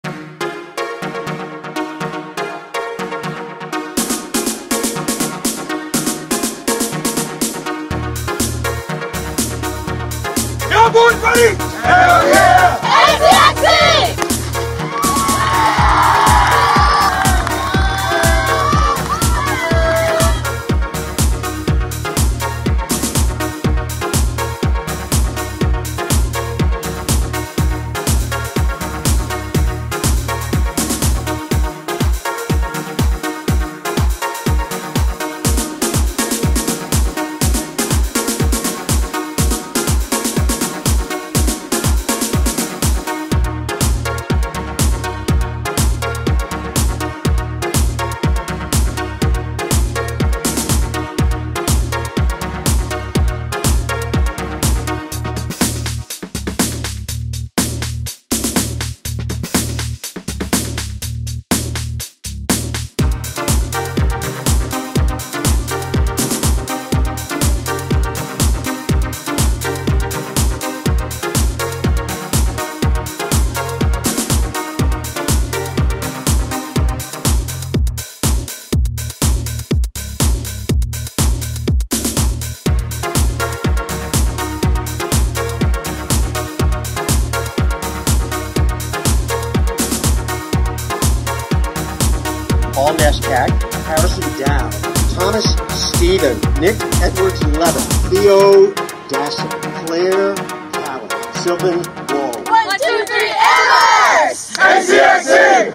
Cross Country: NESCAC Championship Highlights (10/28/23)
Highlights from Amherst College competing at the NESCAC Championships on Saturday, October 28, 2023, in Amherst, Massachusetts.

The Mammoths finished second as a team in both the men's and women's races!